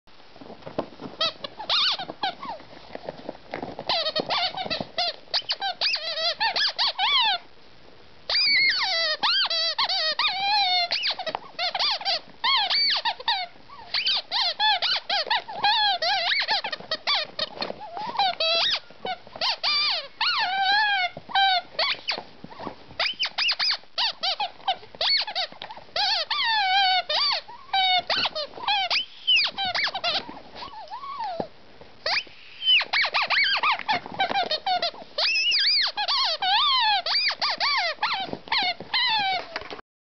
Chimp Tzu
Chimp and shih tzu go at it